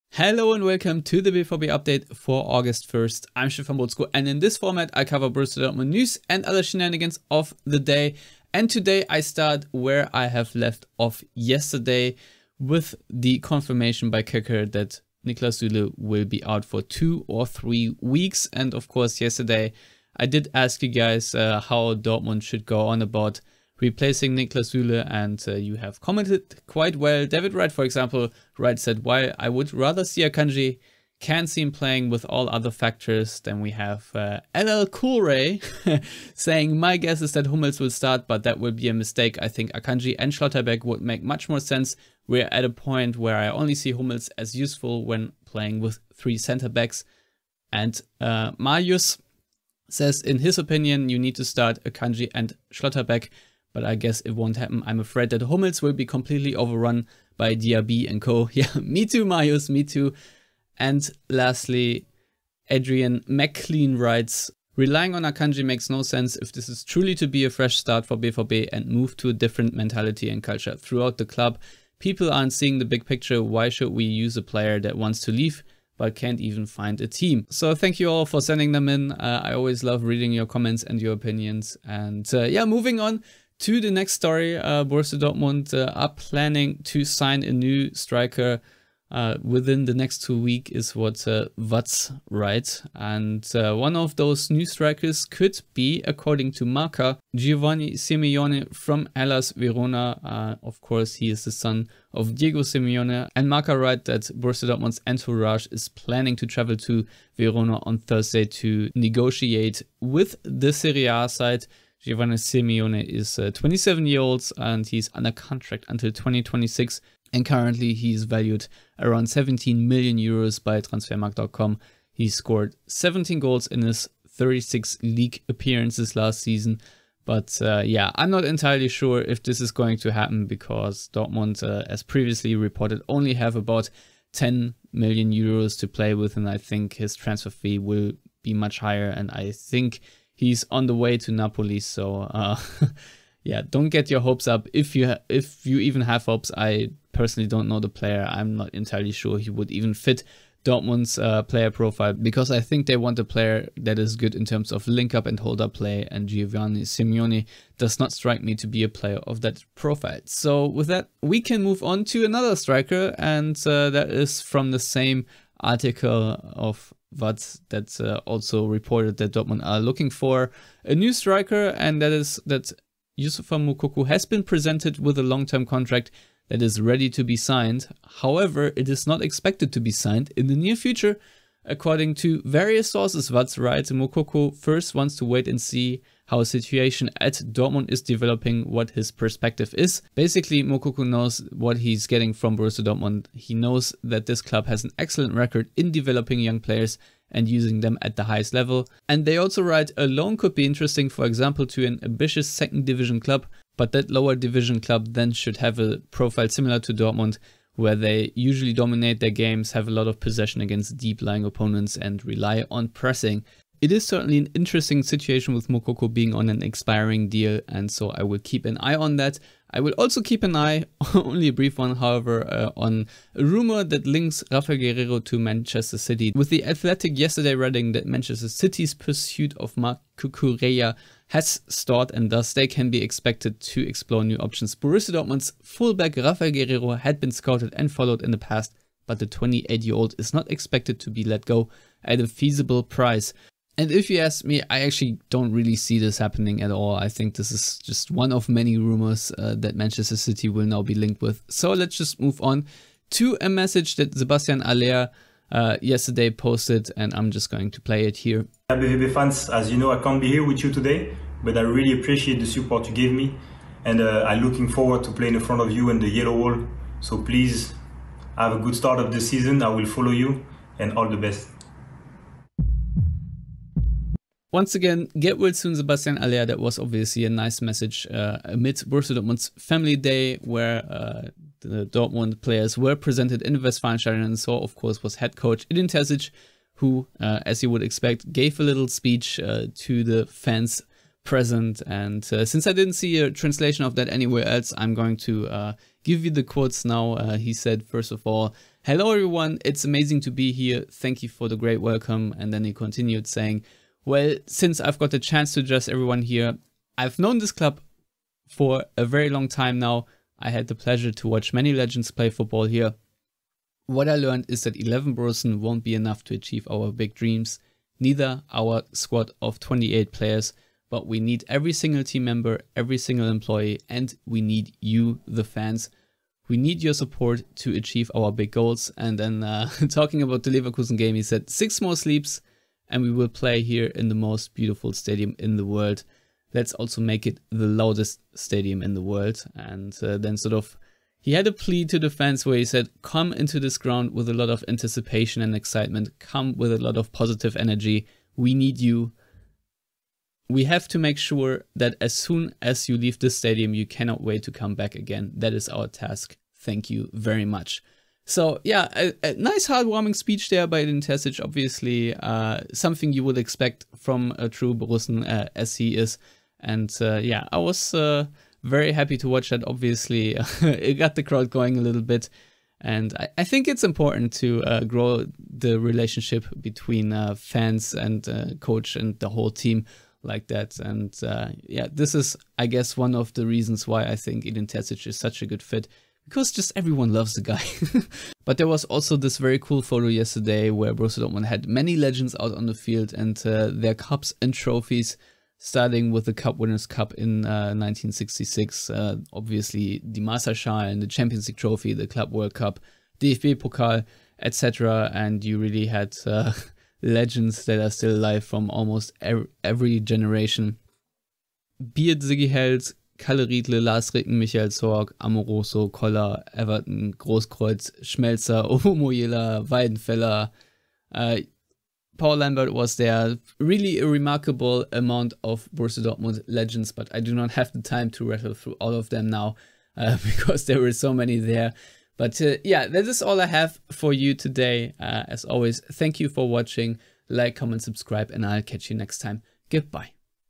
BVB Update 01.08.2022 | Terzic: "We need YOU!"
The BVB Update is a round-up of Borussia Dortmund news, quotes and shenanigans.

Today, more on Niklas Süle, BVB's intention to sign a striker, Youssoufa Moukoko's reluctance to sign, a message from Sebastien Haller, a speech from Edin Terzic and many BVB legends in one picture.